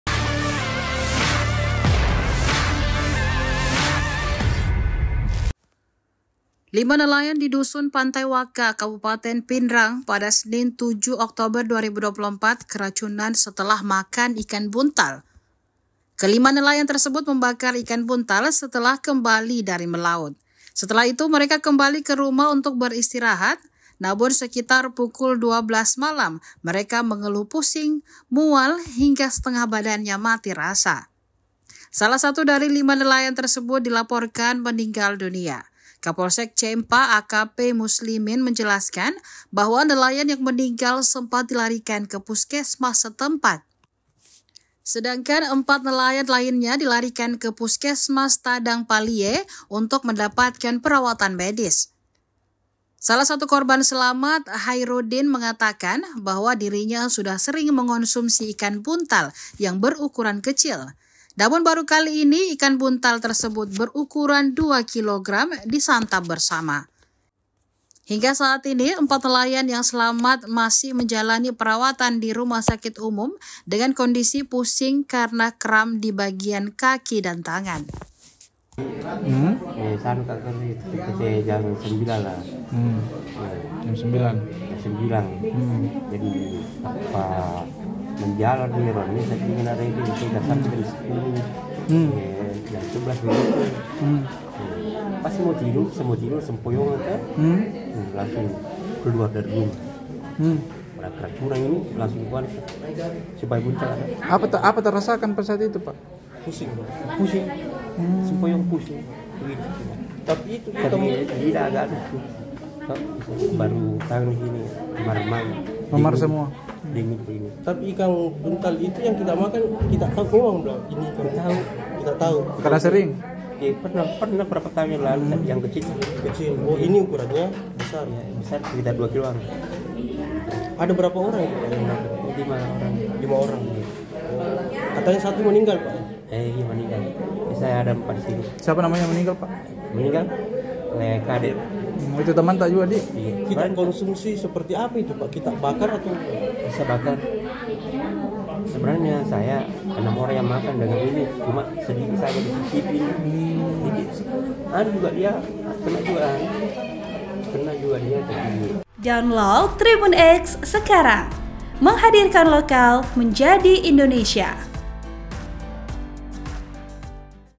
Cerita Korban Selamat Tragedi Ikan Buntal di Pinrang, Terbangun Tengah Malam Gegara Kesemutan
TRIBUN-TIMUR.COM - Lima nelayan di Dusun Pantai Wakka, Kabupaten Pinrang pada Senin (7/10/2024), keracunan setelah makan ikan buntal.

Kelima nelayan tersebut membakar ikan buntal setelah kembali dari melaut.

Setelah itu mereka kembali ke rumah untuk beristirahat, namun sekitar pukul 12.00 malam mereka mengeluh pusing, mual hingga setengah badannya mati rasa.

Salah satu dari lima nelayan tersebut dilaporkan meninggal dunia.

Kapolsek Cempa, AKP Muslimin, menjelaskan bahwa nelayan yang meninggal sempat dilarikan ke puskesmas setempat.

“Korban sempat mendapatkan perawatan di puskesmas sebelum akhirnya menghembuskan napas terakhir,” ujarnya saat ditemui di lokasi kejadian, Selasa (8/10/2024).

Sedangkan empat nelayan lainnya dilarikan ke Puskesmas Tadangpalie untuk mendapatkan perawatan medis.

“Keempat nelayan tersebut kemudian dirujuk ke RS Lasinrang untuk penanganan lebih lanjut,” tambah Muslimin.

Salah satu korban Selamat, Haerudin mengatakan bahwa dirinya sudah sering mengonsumsi ikan buntal yang berukuran kecil, namun kali ini ikan buntal tersebut berukuran 2 Kg.

"Sebenarnya sering dimakan ikan buntal, tapi yang sering memang dulu itu ukuran kecil. Barusan ini saya makan ikan buntal besar, sekitar 2 Kg itu," ujarnya.

Hingga saat ini, empat nelayan yang selamat masih menjalani perawatan di RSU dengan kondisi pusing serta kram di bagian kaki dan tangan.

Naskah : Rasni Gani / Astri Rustan (Mahasiswa Magang UMI)
Narator:  Rasni Gani
Editor Video: Hasma Nur Qadri

(TRIBUN-TIMUR.COM)